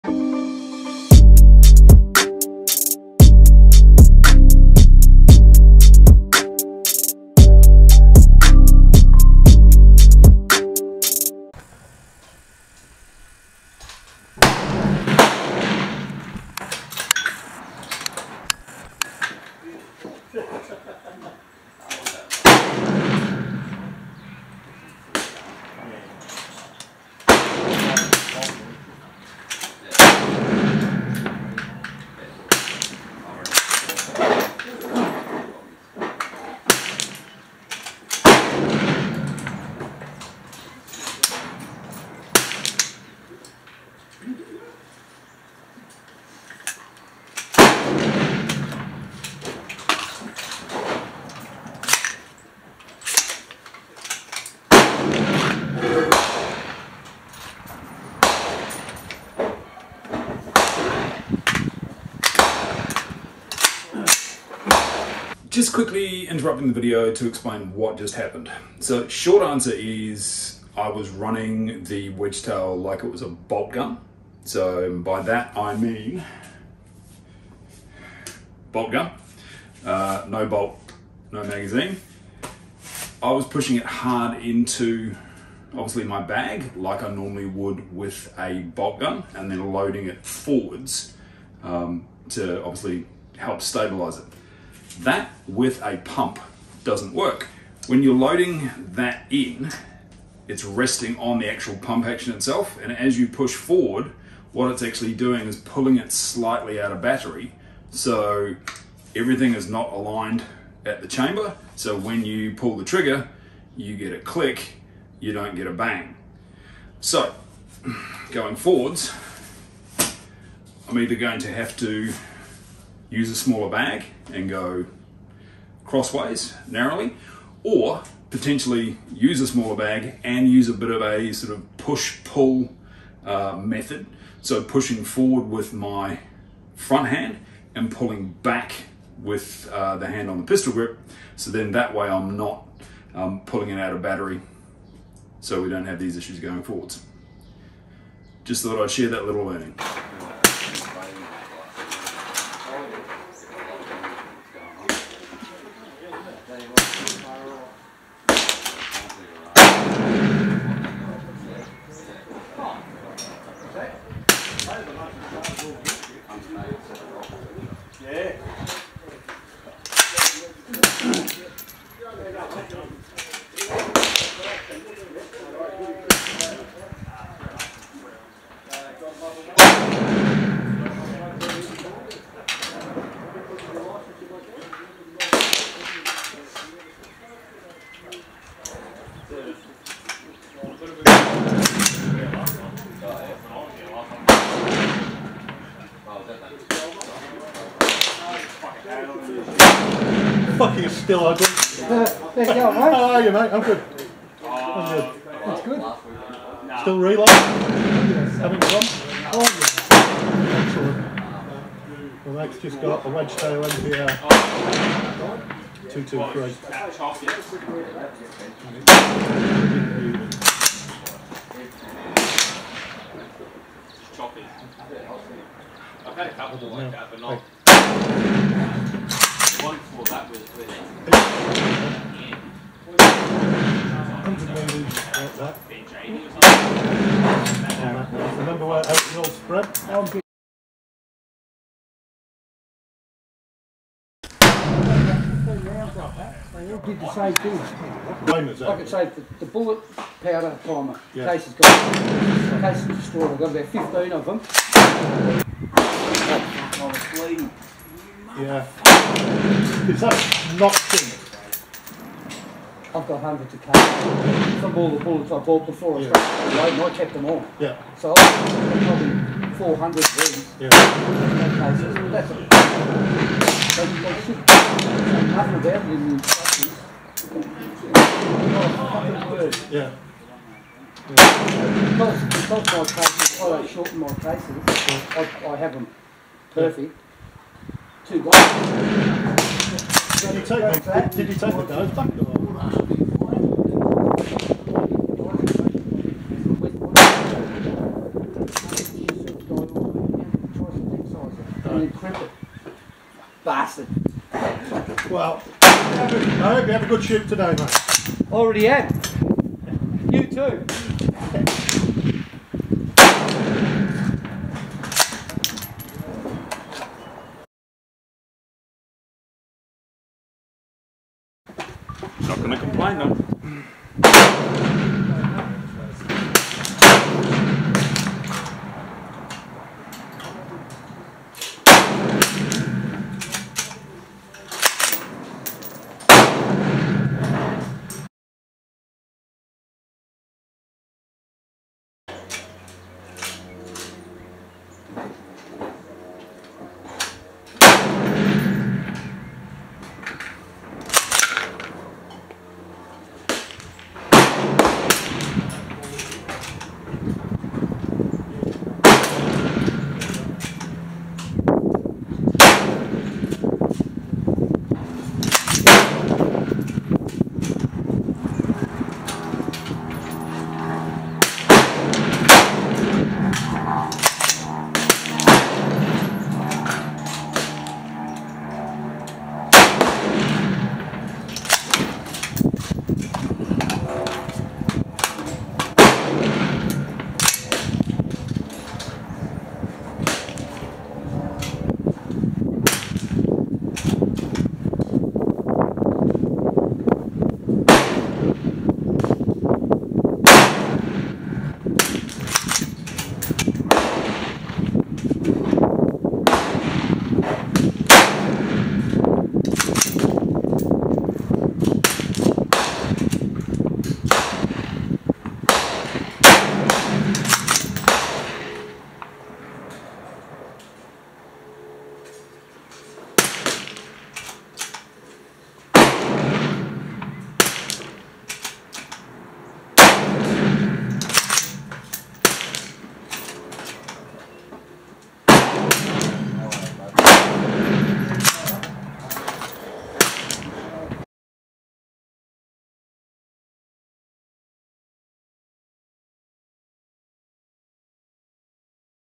Wedgetail MPR Mini 223 - Practical Accuracy Test
I thought I'd try something different, as opposed to just seeing if it'll group off a bench, so I put a paper rabbit at 25m, a paper goat at 50m, and a paper boar at 75m, with 2 inch splatter targets on each, and shot off the tripod with a bag.
Bear in mind the zero is at 100m, so the first shot or two at each are off as I worked out the holds, and I learned that a pump needs to be run differently from a bolt gun on a tripod. This is my first pump action, so mistakes will be made, and I'll happily tell you about them as I make them.
I was using Fiocchi 55gn EPN, and am pretty happy with the results.
A lot more to come with this rifle, as damn it's fun to shoot !!